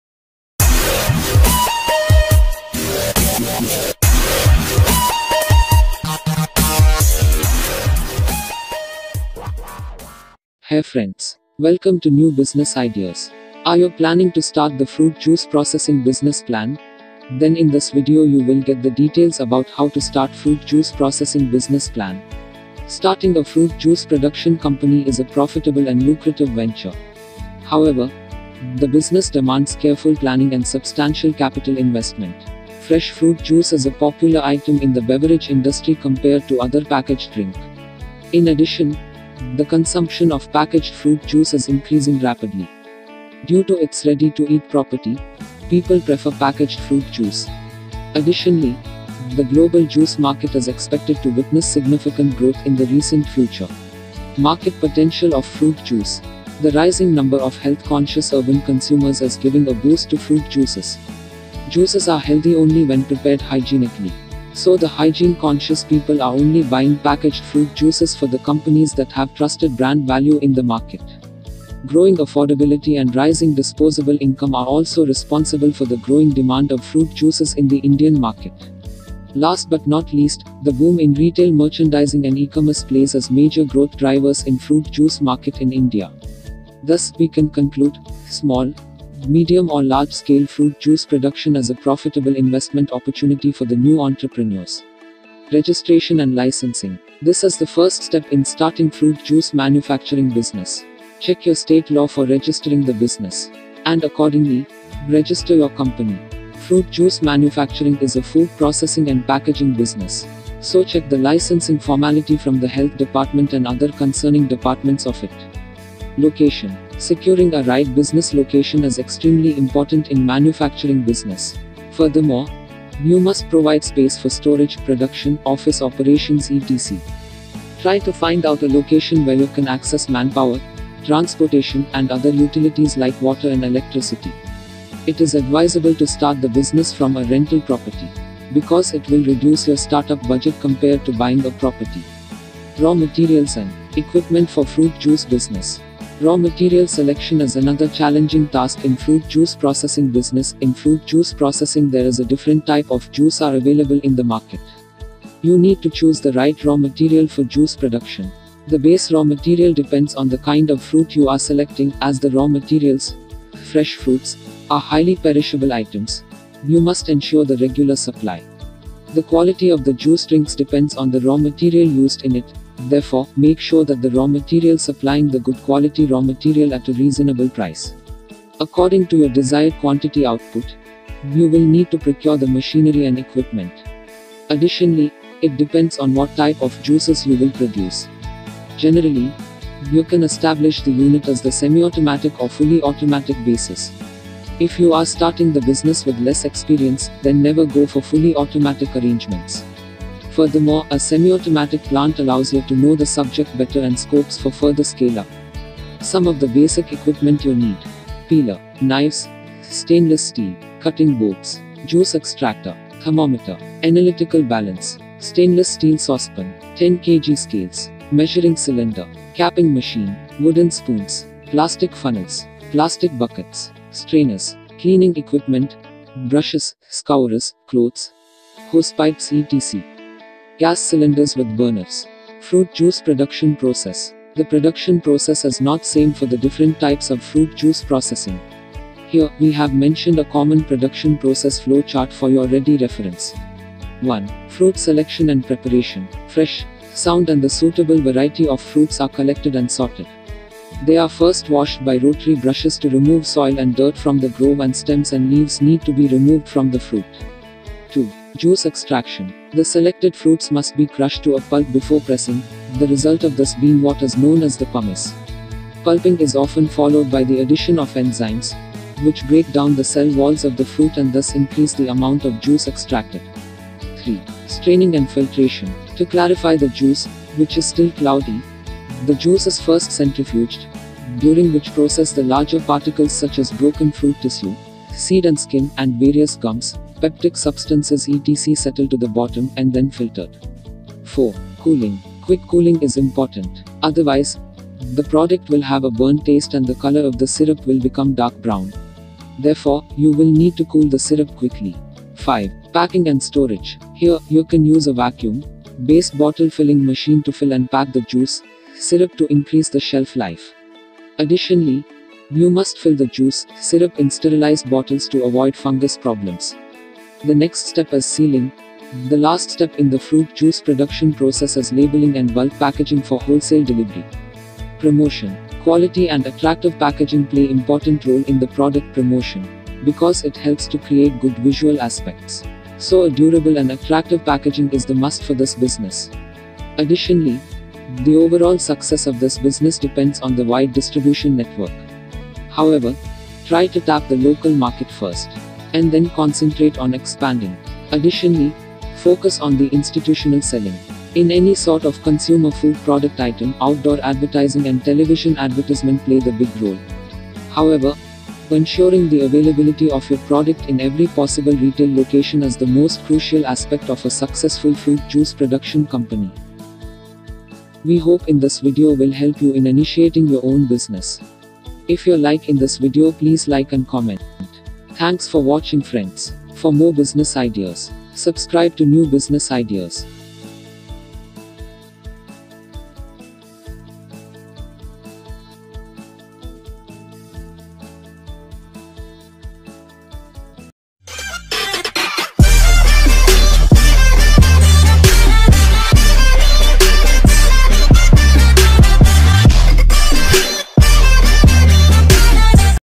Fruit Juice Manufacturing Business | Most Demanded Business ideas
Hai friends
Are you planning to start the fruit juice processing business plan, then in this video you will get the details about how to start fruit juice processing business plan.